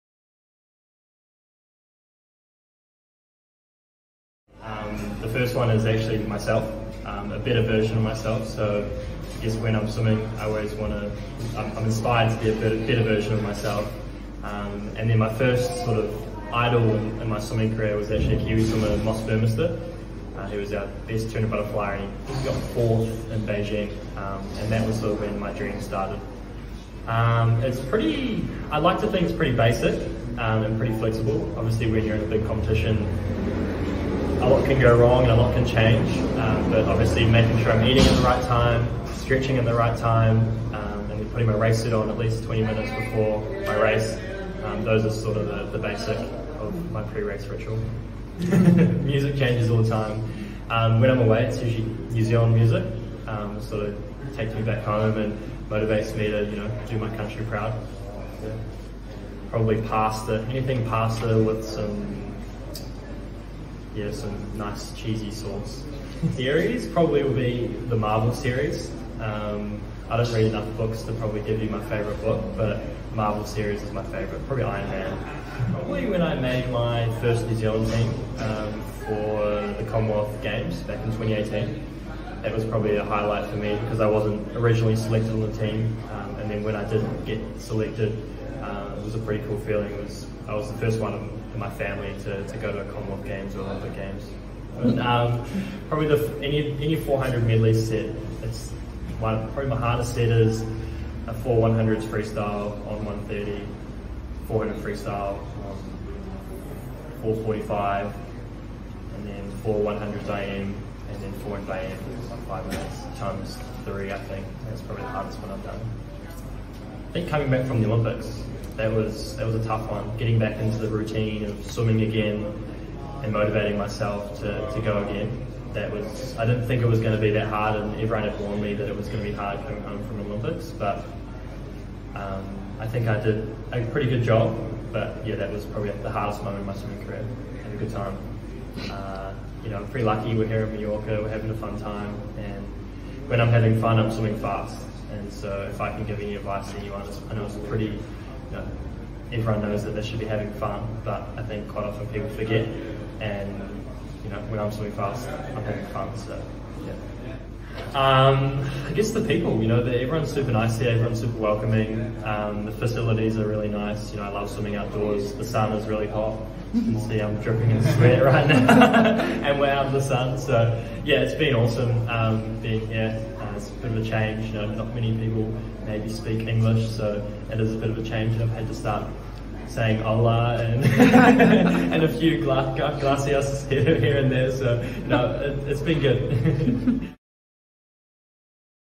10 of the BEST - Lewis Clareburt
We interview Lewis Clareburt, World Championshop Medalist. Lewis is training for the Commonwealth Games in our pool with the New Zealand National Team and if we have discovered anything it is that he is in love with Mallorca. Learn more about him in this video!